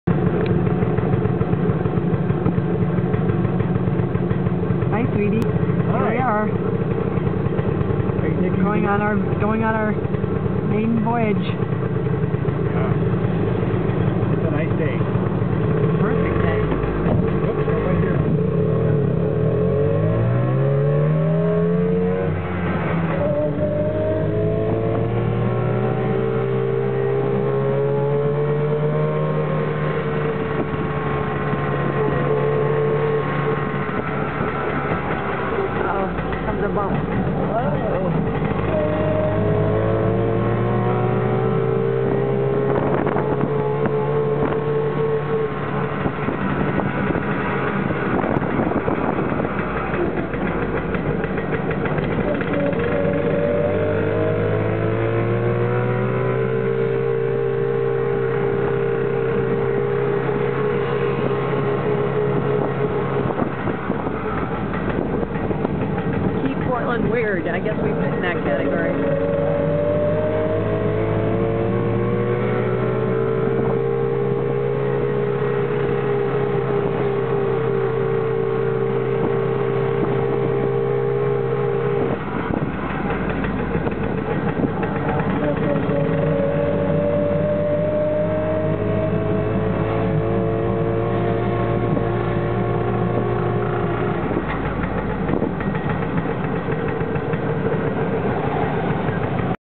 Our Messerschmitt's 1st Tandem Spring Outing
Lisa's first ride. Hazy-Sunny but cool, 55*f,  The day before Easter, Spring 2008. March 21st. A great day for a Saturday microcar drive!.